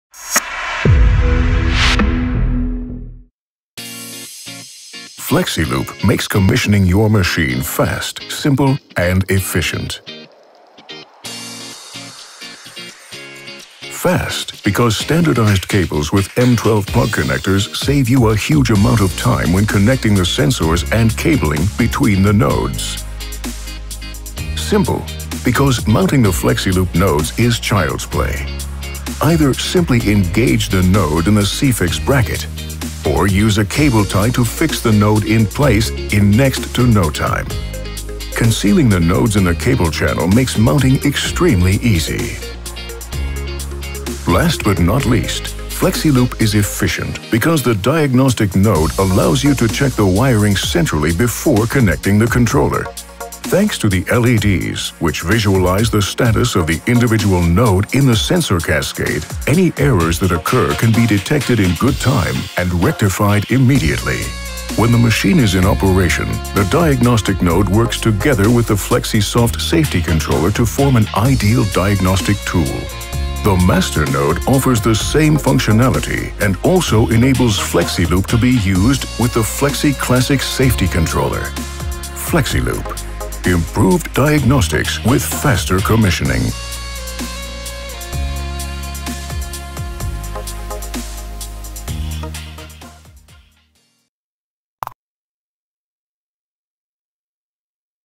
Flexi Loop from SICK: Advantages in series for faster machine commissioning | SICK AG
With Flexi Loop a safe series connection of up to 32 safety sensors while maintaining the highest level of safety is possible. Regardless of the manufacturer, any combination of safety switches and safety sensors with OSSD outputs can be used. Such a system set-up also guarantees the continuous diagnosis of all door switches, emergency stop push-buttons and sensors. Used in connection with Flexi Soft, the entire safety application can be tailored to meet the customer’s needs – cost-effective, personalized and efficient.